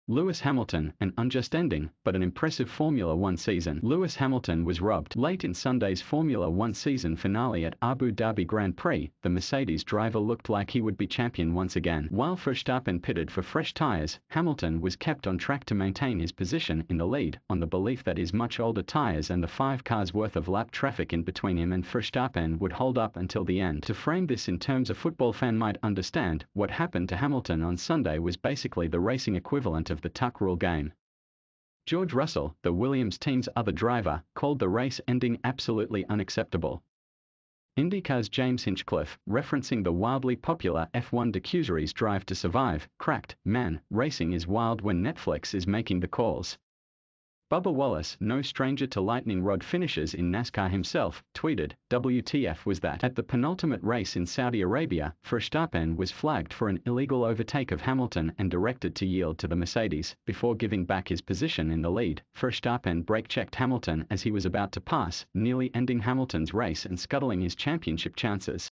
Lewis Hamilton: an unjust ending, but an impressive Formula One season
Lewis Hamilton was robbed.Late in Sunday’s Formula One season finale at Abu Dhabi Grand Prix, the Mercedes driver looked like he would be champion once again. He was leading with a solid gap on Max Verstappen, the Red Bull prodigy with whom Hamilton entered the race atop the standings — setting the stage for a Game 7-style race that would come down to the highest finisher.For Hamilton, though, taking the checkered flag wouldn’t have just broken a points tie with Verstappen this year, it would have further separated him from Ferrari’s Michael Schumacher as Formula One’s first-ever eight-time champion. Or, to put it another way: the sport’s first and only Black driver would have a legitimate claim to undisputed GOAT status.Or at least that was until a late crash brought out the safety car with five laps to go. While Verstappen pitted for fresh tires, Hamilton was kept on track to maintain his position in the lead, on the belief that his much older tires and the five cars’ worth of lapped traffic in between him and Verstappen would hold up until the end.But that math changed when Red Bull team principal Christian Horner radioed race director Michael Masi and sternly advised him to reset the cars to their true running order. Instead of sending all the lapped traffic to the end of the line, which would’ve taken longer than there were laps left to run in the race, Masi waited until the last minute just to shoo away the five cars in between Hamilton and Verstappen — teeing the two rivals up for a sudden-death, single-lap shootout for all the marbles.That late call caught Mercedes completely off guard and left Hamilton totally overmatched against Verstappen when his Red Bull machine was positioned right behind the Mercedes. When the race was restarted, Hamilton could only fend off Verstappen for a few turns before Verstappen shot past and cruised to the first title of his career.To frame this in terms a football fan might understand, what happened to Hamilton on Sunday was basically the racing equivalent of the “Tuck Rule Game.” George Russell, the Williams team’s other driver, called the race ending “absolutely unacceptable.” IndyCar’s James Hinchcliffe, referencing the wildly popular F1 docuseries Drive to Survive, cracked: “Man, racing is wild when Netflix is making the calls!” Bubba Wallace, no stranger to lightning rod finishes in NASCAR himself, tweeted: “Wtf was that?!?”Mercedes, for its part, lodged two formal protests only to see them rejected. But then again, that was to be expected given that they were essentially accusing the refs of making up the rules. (Likely, this is going to court.)Masi had so many better options at his disposal than what he ultimately chose. He could have stopped the race entirely to clear the crash wreckage, which would’ve given Hamilton and Verstappen an opportunity to swap for new tires and set up a fair duel. Or Masi could’ve let the race finish behind the safety car and left the running order unchanged, a tack he took earlier this season when rain overwhelmed the Belgian Grand Prix. Instead, Masi stacked the deck for Verstappen — and mere moments after, Horner conceded on the Sky Sports broadcast that Hamilton’s Mercedes “was too strong today.” After Verstappen crossed the finish line, Horner called it a “miracle.”While Hamilton remained gracious in defeat, congratulating Verstappen and his team on a hard-fought campaign, Verstappen grinned like the cat who swallowed the canary. “Finally, a bit of luck for me today,” he told Sky Sports. It was enough to make you hurl a remote at the TV.Where Hamilton came from humble, middle-class beginnings, Verstappen is the son of an ex-Formula One driver. He wasn’t just bred to be world champion, but to be the youngest one ever, starting in go-karts at age 4 before landing a full-time F1 seat at the record age of 17. After struggling to keep pace with the Mercedes team for the past six years, Red Bull finally furnished Verstappen with a car that could take the fight to Hamilton. Verstappen’s pit crew is the fastest in the business. His teammate, Sergio Perez, is strong enough to provide cover.Along with those advantages, Verstappen is also massively talented, tenaciously competitive and a habitual line stepper. All season he got away with racing Hamilton overzealously. At the penultimate race in Saudi Arabia, Verstappen was flagged for an illegal overtake of Hamilton and directed to yield to the Mercedes; before giving back his position in the lead, Verstappen brake-checked Hamilton as he was about to pass, nearly ending Hamilton’s race and scuttling his championship chances.Mario Renzi-Formula One/Formula One via Getty ImagesIn the lead-up to Abu Dhabi, the burning question was whether Verstappen would intentionally collide with Hamilton on the first corner to guarantee himself the championship. (As the “victor” in the Belgian Grand Prix race, Verstappen technically led Hamilton in the standings by virtue of having won nine races to his